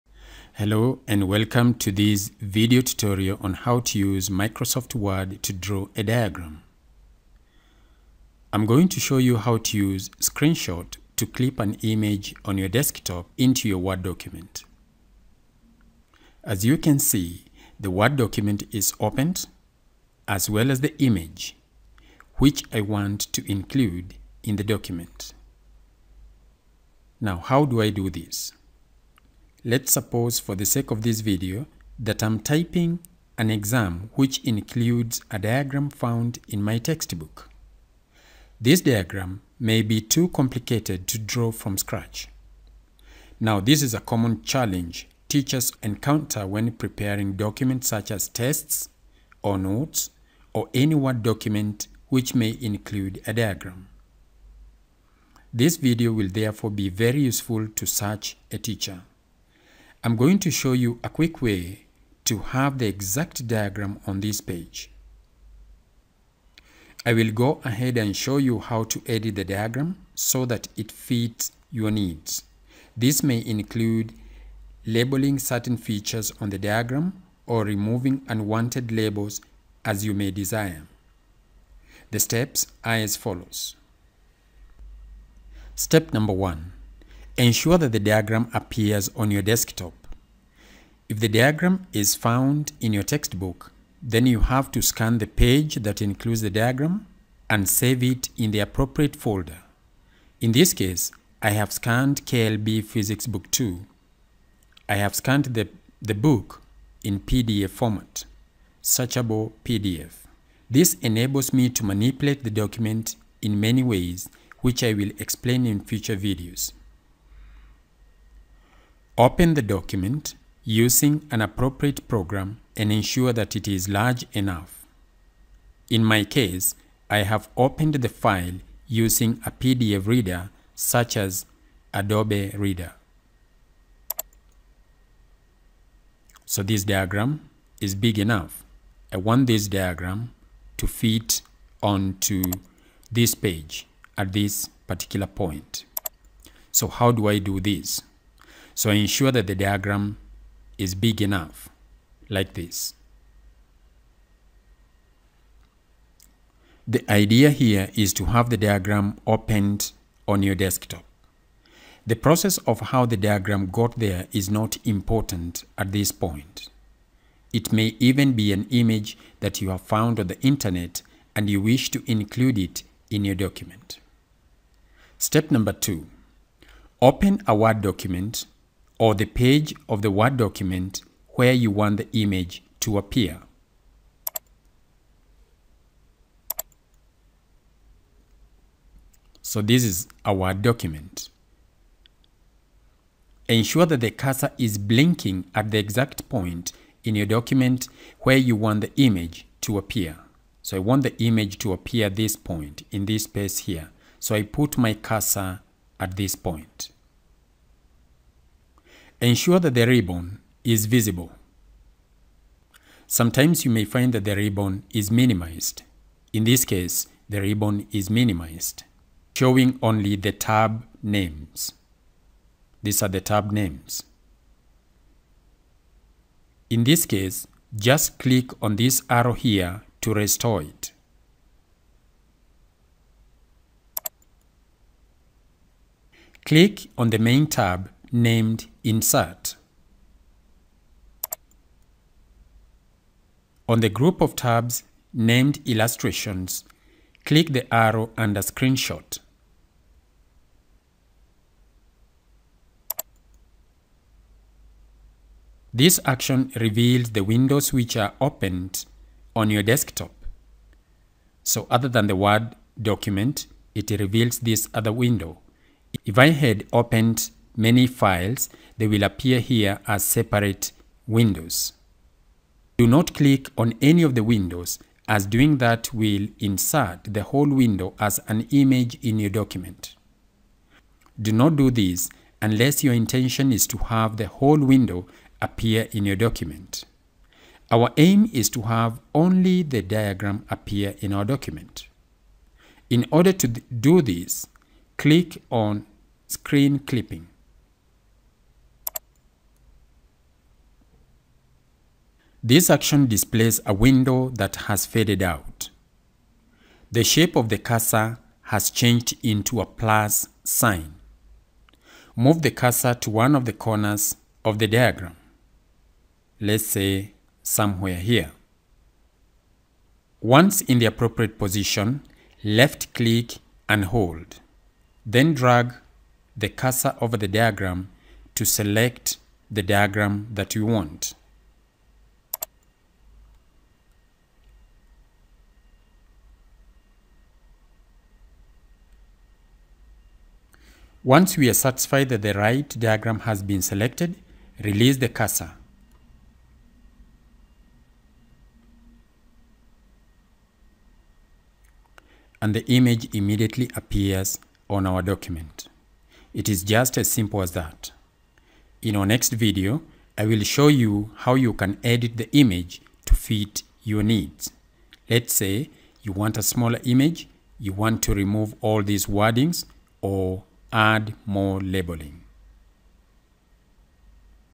HOW TO USE MICROSOFT WORD TO DRAW A DIAGRAM
Many teachers and educators find the skill of drawing diagrams on the documents very challenging. Since many of these diagrams are usually copied or traced from another document such as a textbook, I show you how to do this using Microsoft Word.